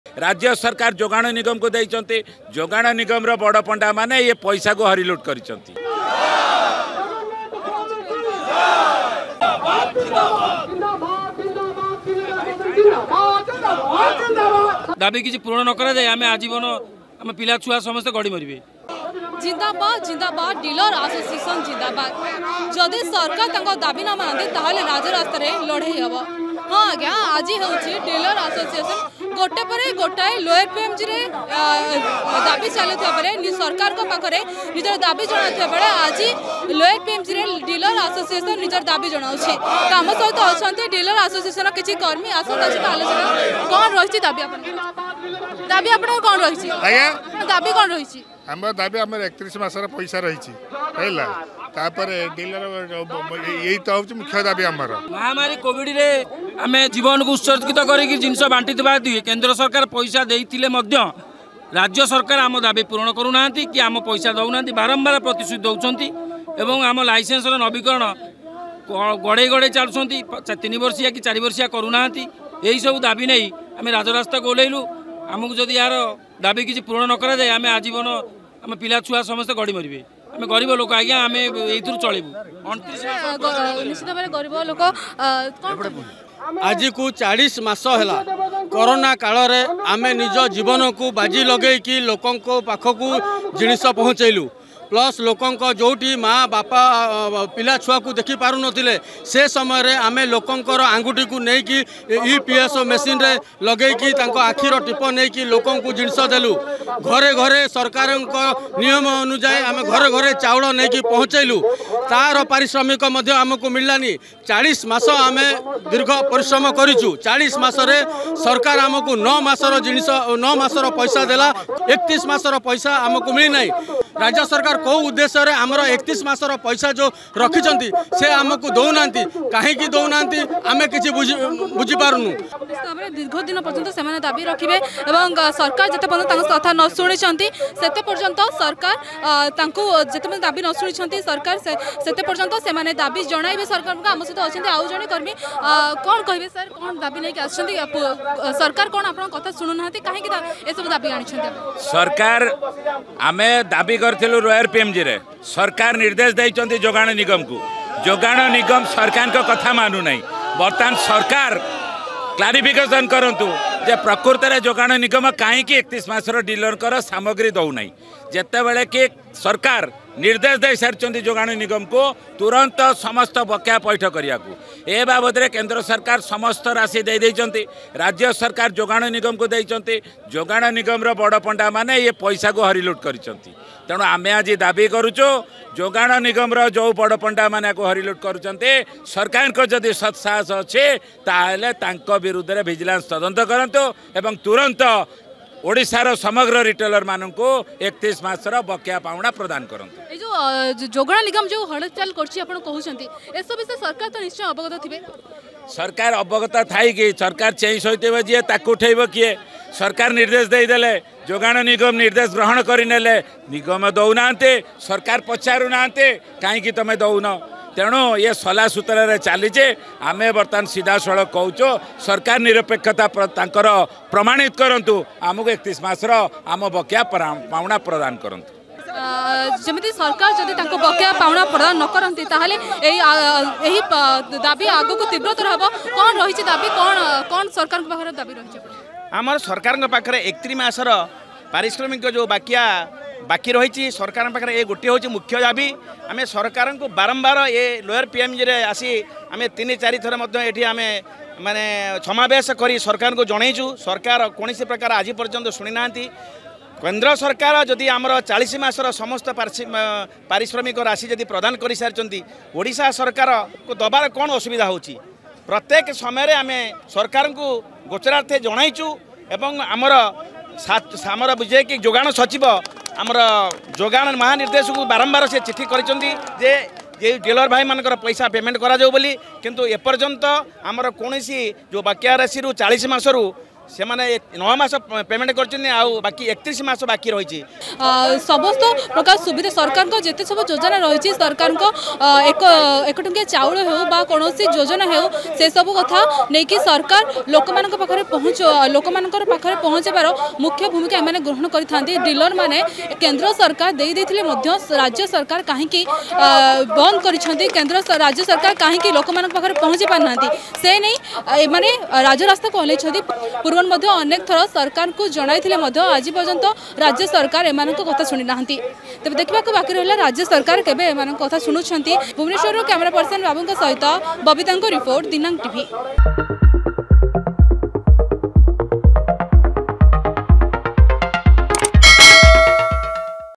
ଡିଲରଙ୍କ ଟଙ୍କା ଖାଇଥିବା ବଡ଼ପଣ୍ଡା କିଏ ? || Dinanka Tv
ଜଗନ୍ନାଥ ସ୍ୱାମୀ କି ଜୟ ନାରାରେ କମ୍ପିଲା ରାଜଧାନୀ । ଦାବି ଜଣାଇ କମ୍ପିଲା ଡିଲର ଆସୋସିଏସନ ।